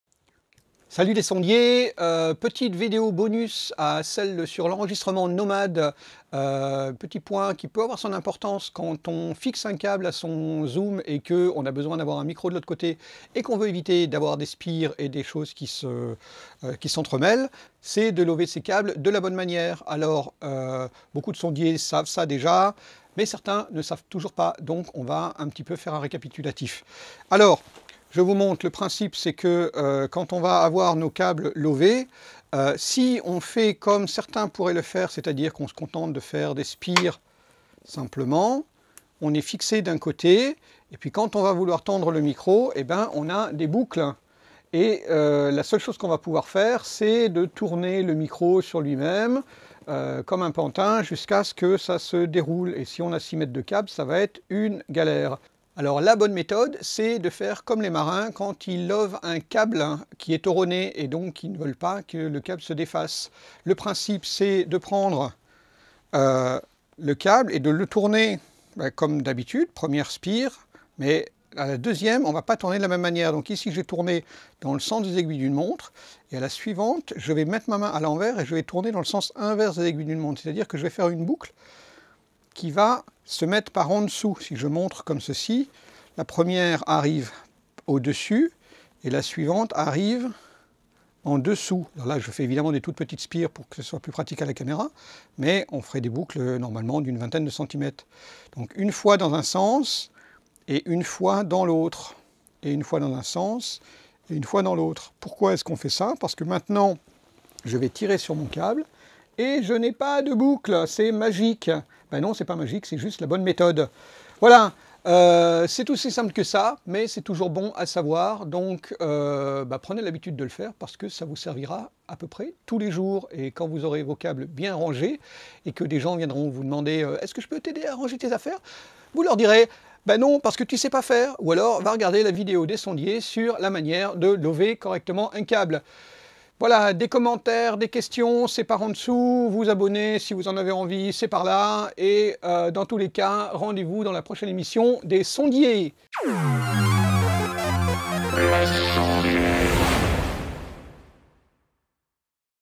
Enrouler (lover) correctement un cable audio
En 2 minutes 30, Blast vous montre comment enrouler (lover) correctement un cable quand on est un bon sondier.

Parce que le mauvais sondier, il prend un cable, il le love, bon.
Par contre le bon sondier, il prend un cable, il le love, mais c'est un bon sondier...